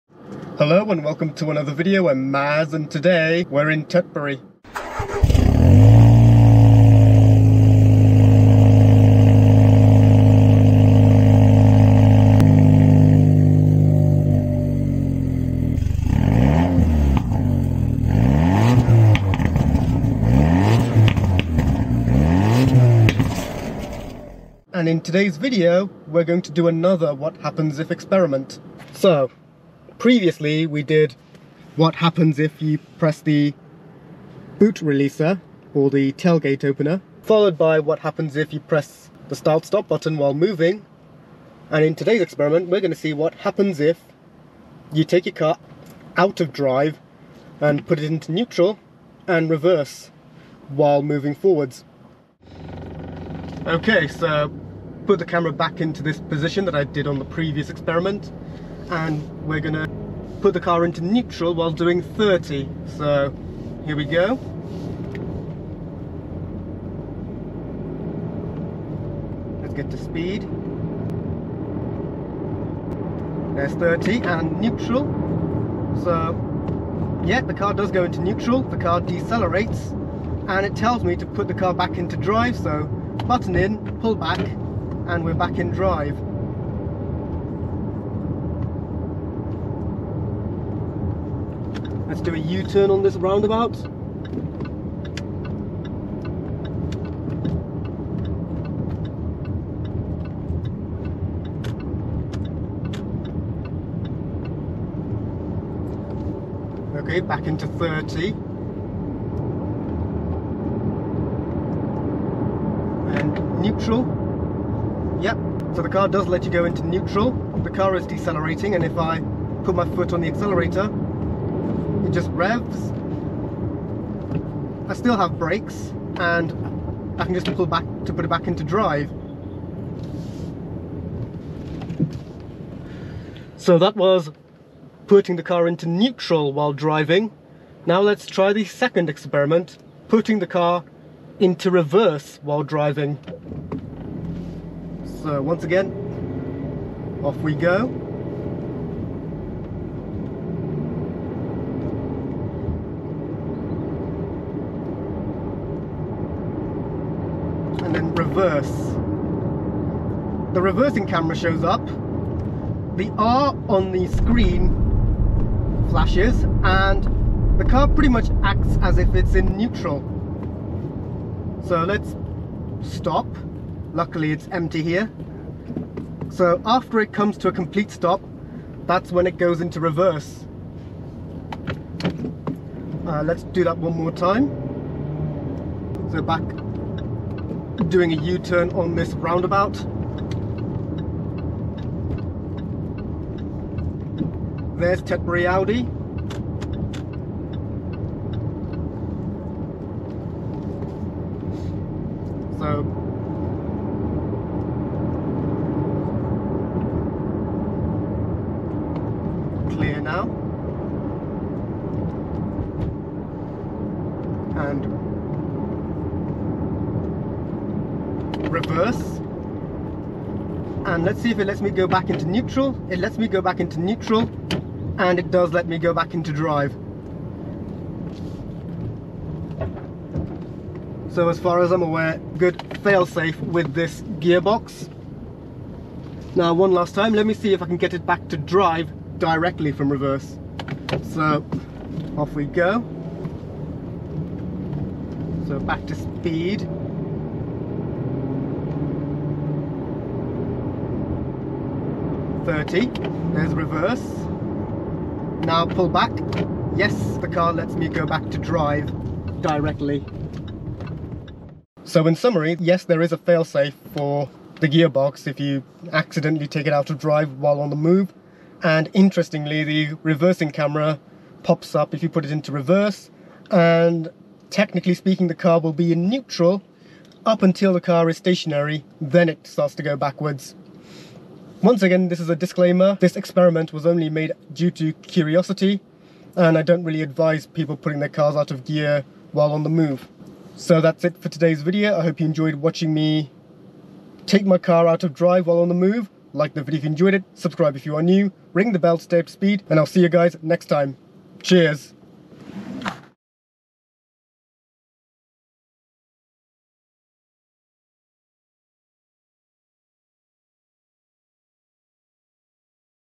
What Happens If you put your car in Reverse (& Neutral) WHILE driving forwards?
Once again, another 'what happens if' experiment, this time regarding the transmission